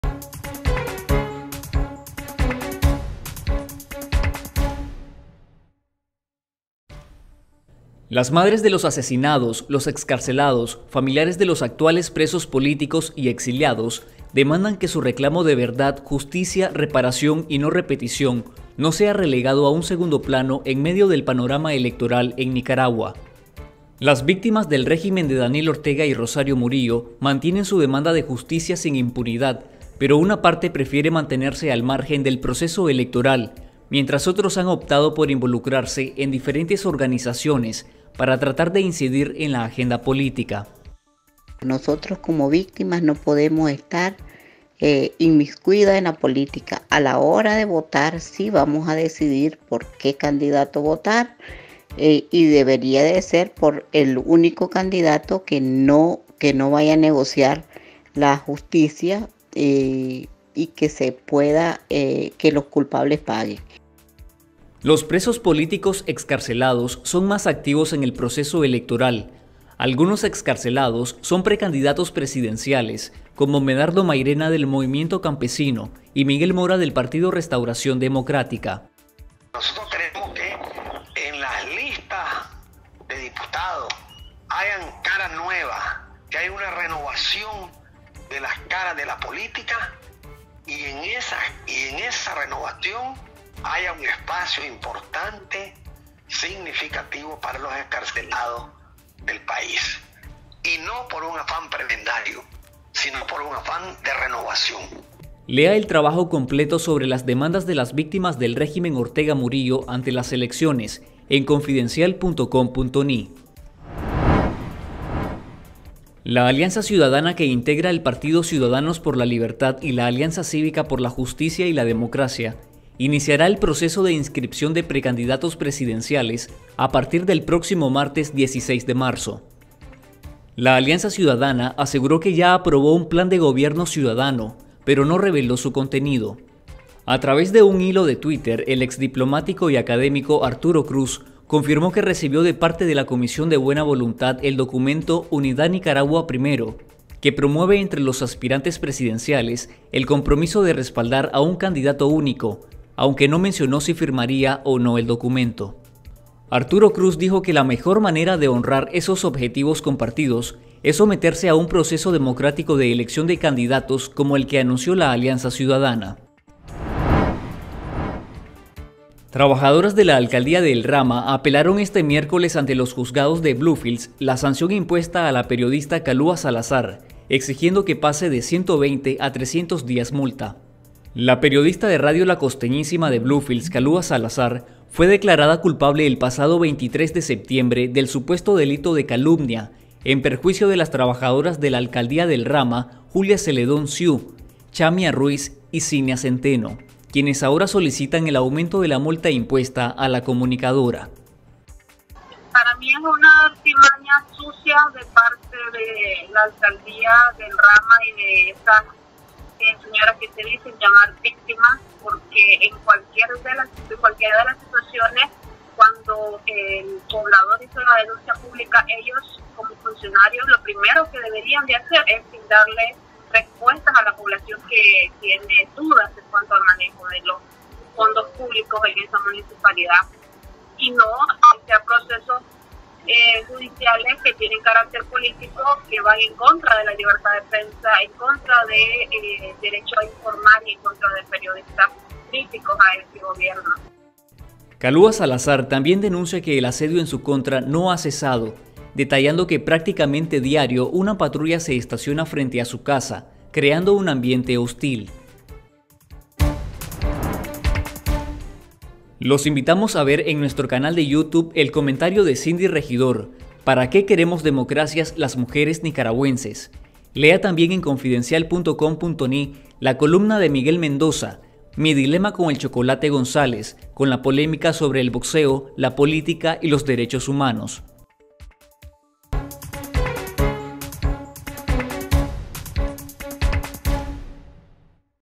Las víctimas de la represión hablan sobre elecciones 2021: “¡Que no haya impunidad!”
Las madres de los asesinados, los excarcelados, familiares de los actuales presos políticos y exiliados, demandan  que su reclamo de verdad, justicia, reparación y no repetición, no sea relegado a un segundo plano en medio del panorama electoral en Nicaragua.

- Alianza Ciudadana anuncia inscripción de precandidatos.
- Solicitan aumento de multa en contra de la periodista Kalúa Salazar.

Recomendación del día | Comentario de Cindy Regidor, para que queremos democracia las mujeres nicaragüenses. La columna de Miguel Mendoza “Mi dilema con el Chocolate Gonzalez”, con la polémica sobre el boxeo, la política y los derechos humanos.

Este es nuestro Reporte de Noticias.